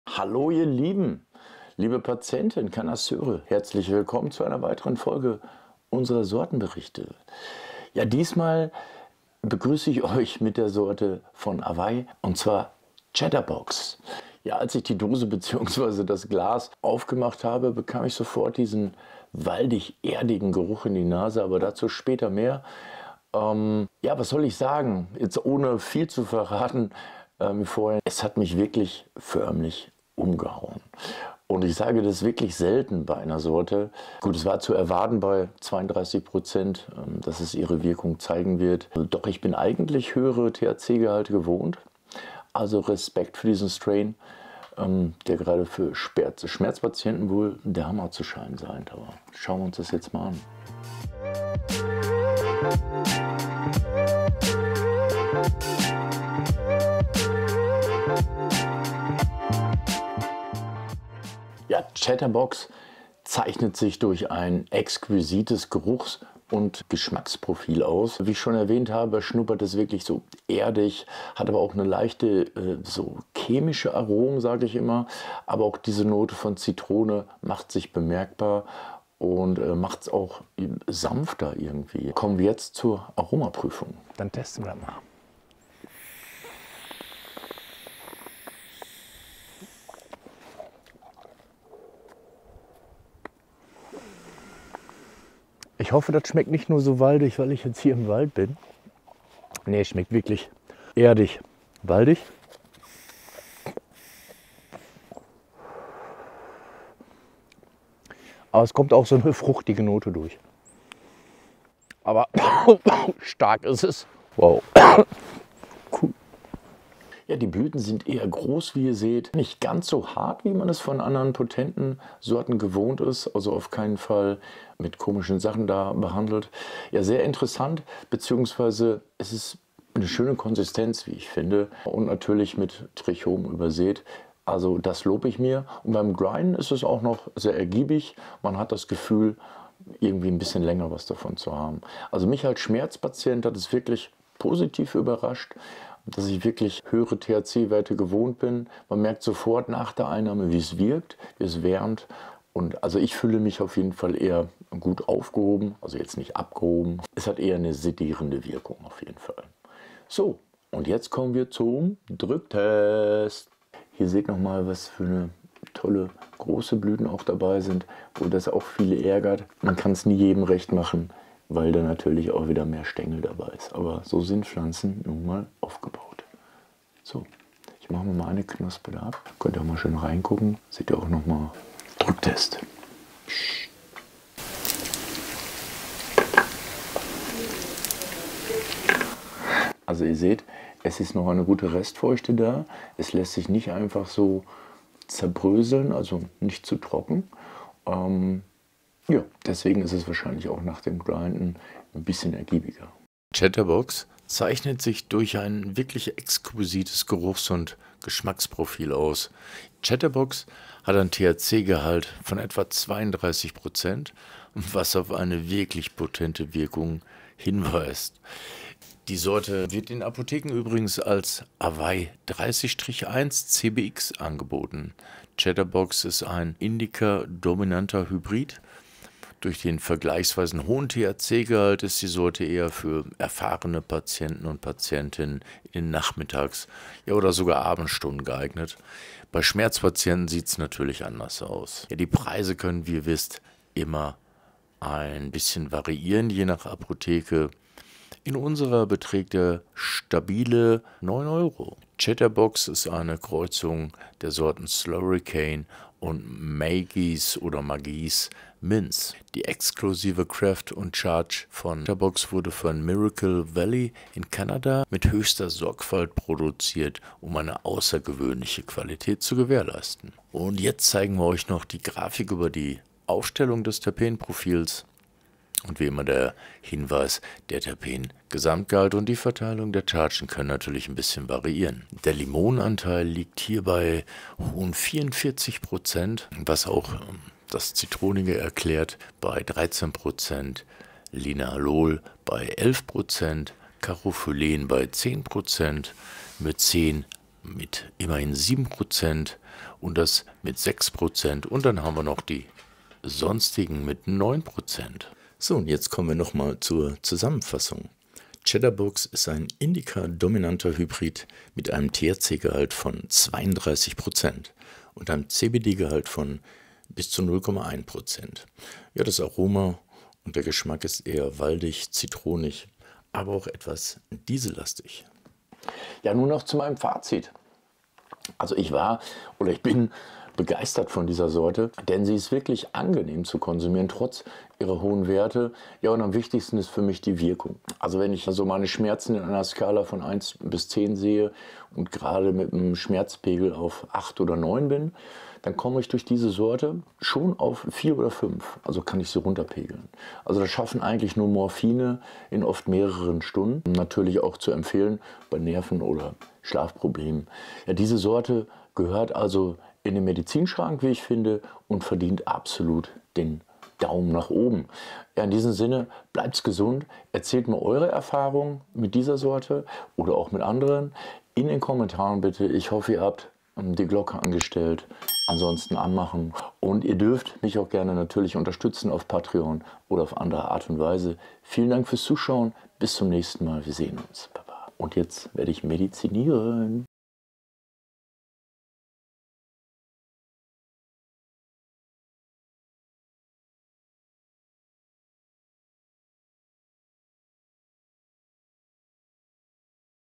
Der medizinische Sortenbericht - Chatterbox - avaay 30/1 CBX
Willkommen zurück, liebe Cannasseure, Patienten und Freunde der grünen Medizin! Wir hoffen, ihr seid fantastisch ins neue Jahr gekommen. Heute bringen wir euch eine ganz besondere Episode unserer beliebten Sortenberichte – diesmal steht Chatterbox im Rampenlicht, und glaubt uns, diese Sorte ist eine Klasse für sich.

Stellt euch vor, ihr öffnet die Verpackung und werdet sofort von einem intensiven Duftgemisch aus erdigen Waldnoten und einem Hauch von Fruchtigkeit begrüßt. Chatterbox hat mich, und das sage ich nicht leichtfertig, absolut umgehauen. Trotz meiner Erfahrung mit hohen THC-Werten, hat die Potenz dieser Sorte mit 32% THC meine Erwartungen übertroffen. Ein echtes Highlight, besonders für Schmerzpatienten.

In dieser Folge nehmen wir euch mit auf eine Reise durch das einzigartige Aroma- und Geschmacksprofil von Chatterbox. Wir erkunden die großen, trichomreichen Blüten und ihre beeindruckende Konsistenz. Aber es geht nicht nur ums Aussehen; die Wirkung dieser Sorte auf Schmerzen ist bemerkenswert – eine sanfte, aber effektive Linderung, die überwältigt, ohne zu überfordern.

Entdeckt mit uns, was Chatterbox so besonders macht. Von seiner genetischen Abstammung, einer Kreuzung aus "Slurricane" und "Margy's Mints", bis hin zur sorgfältigen Produktion in Kanada, wir decken alles ab. Wir werfen auch einen Blick auf das Terpenprofil, das diese Sorte nicht nur geschmacklich, sondern auch therapeutisch zu einem wahren Kraftpaket macht.

Zum Schluss teile ich meine persönlichen Erfahrungen mit Chatterbox. Es ist eine Sorte, die angenehm zu konsumieren ist und deren Wirkung mich tief beeindruckt hat. Von starken Schmerzen zu spürbarer Erleichterung – ein Effekt, der oft Stunden dauert und hier fast sofort eintritt. Eine Sorte, die in jedem Medizinschrank seinen Platz finden sollte.

Bleibt dabei, unterstützt uns und lasst uns eure Erfahrungen wissen.

Vergesst nicht, die Glocke zu aktivieren, damit ihr keine unserer Folgen verpasst. Bis zum nächsten Mal, bleibt gesund und lasst euch inspirieren!